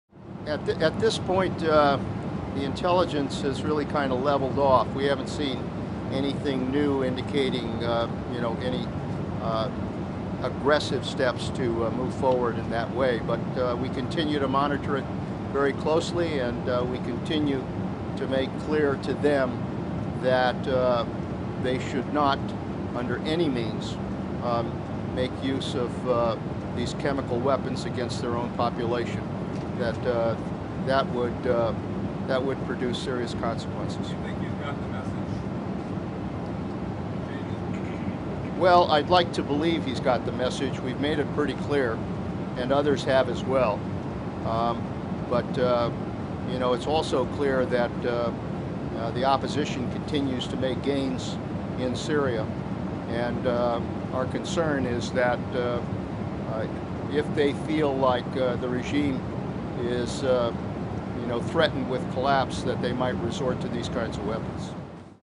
Panetta No new signs Syria prepping WMD
During a flight to Kuwait, Defense Secretary Leon Panetta was asked about intelligence reports that the Syrian regime was prepping chemical weapons for use against its own people; He replied,